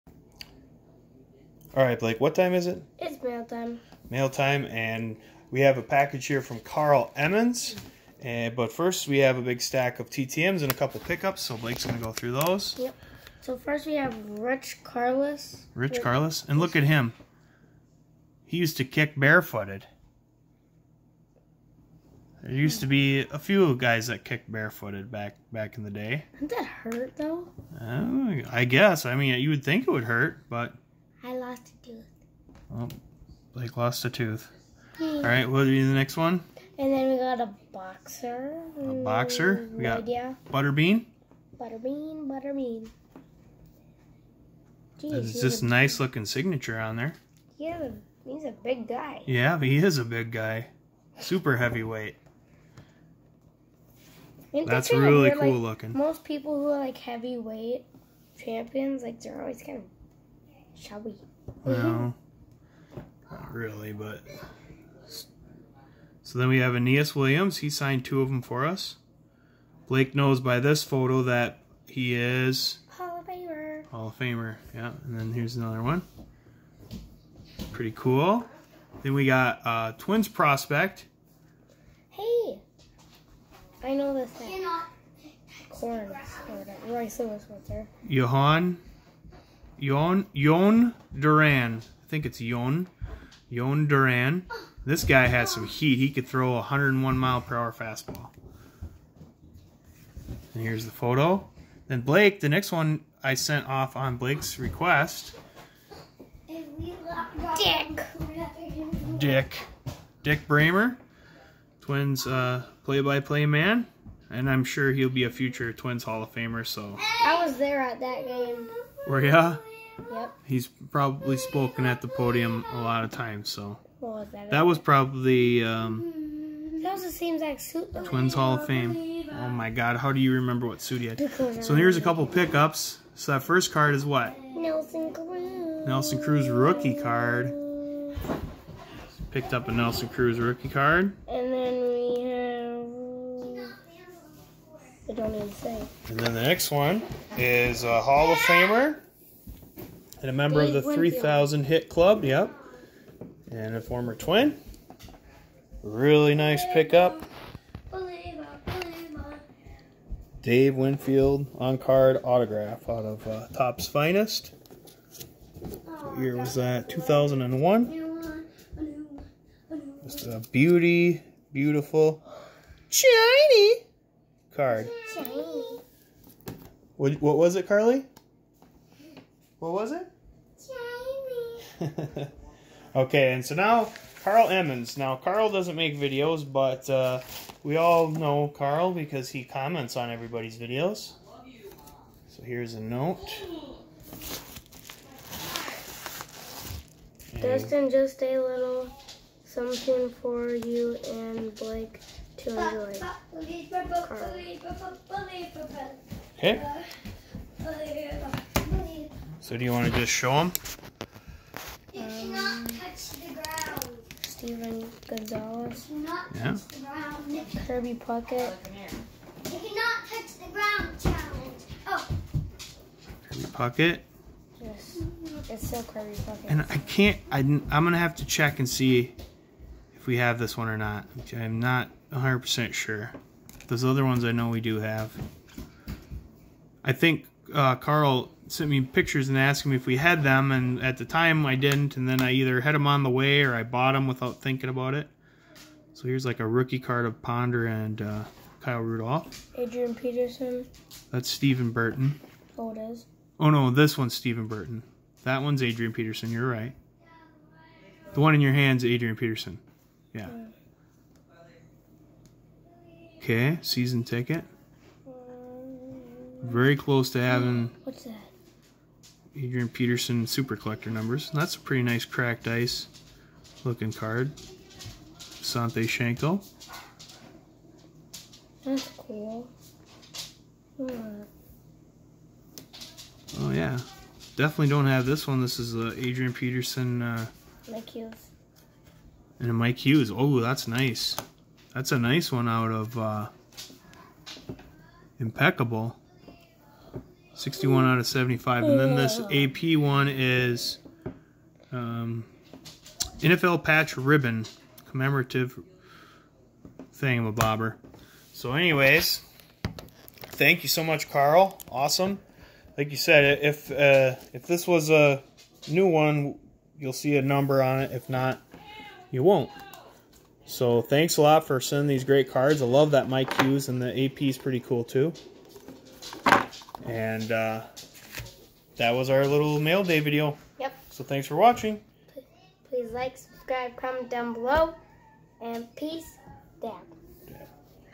Dave Winfield Autograph, TTMs, & Karl Emmons Mail!!!
Thank you so much Karl, for the awesome care package. Blake shows off some of our recent TTMs and a sweet PC pick up of a baseball Hall of Famer!!!